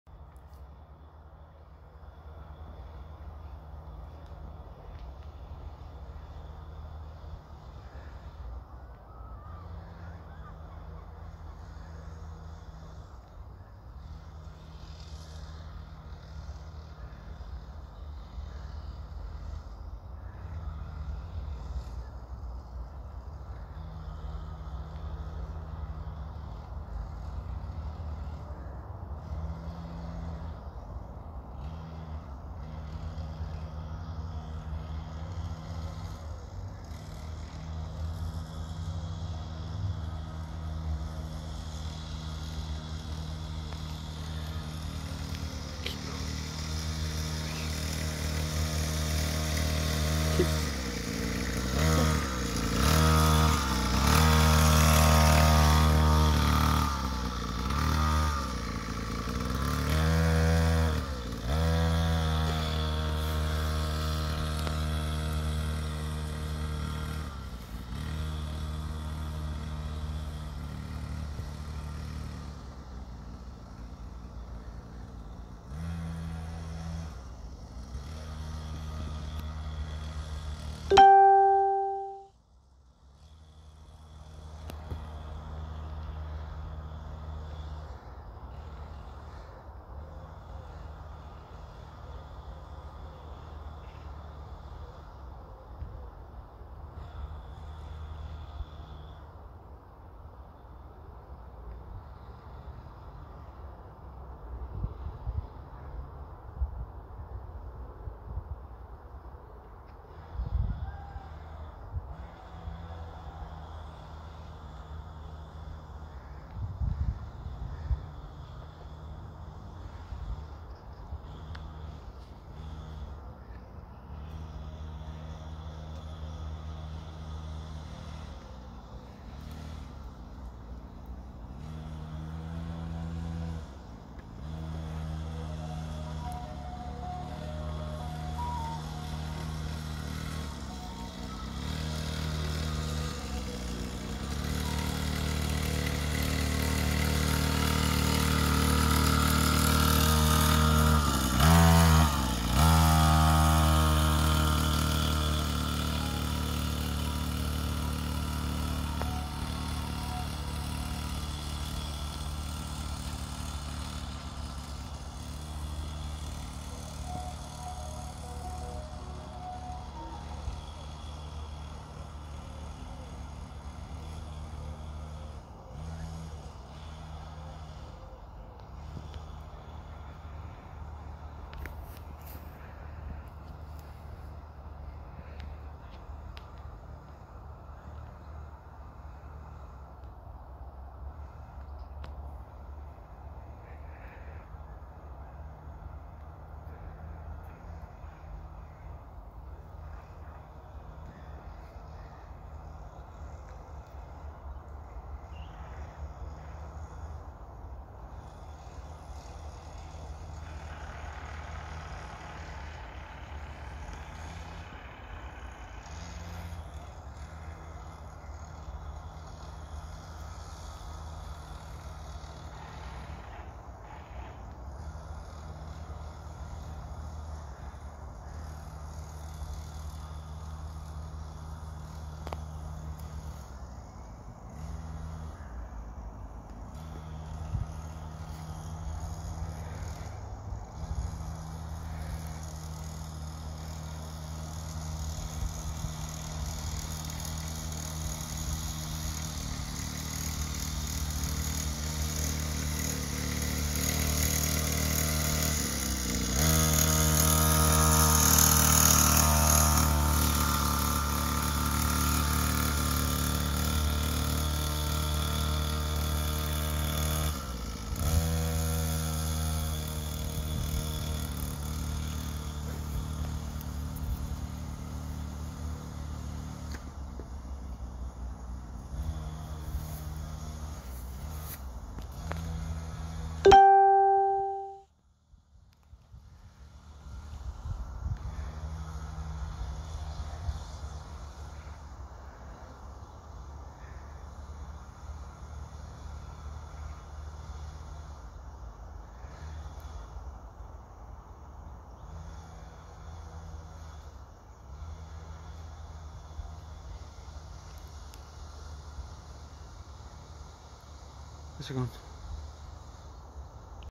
Chloe McHendry XTM pro rider 50cc
My daughters third shot of going her dirtbike